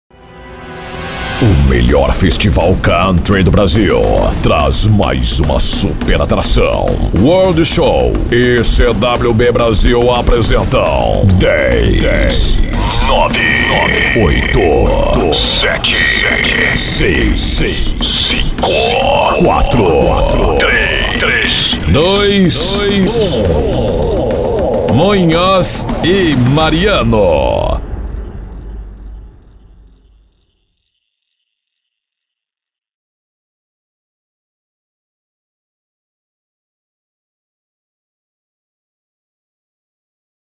ABERTURA DE SHOW MUNHOZ E MARIANO
Locução estilo cinema para abertura de palco do Country Festival.

Home Mix Studio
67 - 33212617 ou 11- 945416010